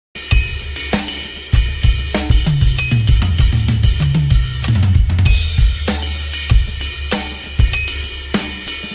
drum grooves and fills with five beat ride pattern
developing drumset grooves and fills using a five beat ride pattern. check out the full video at my YouTube channel @rjrichmusic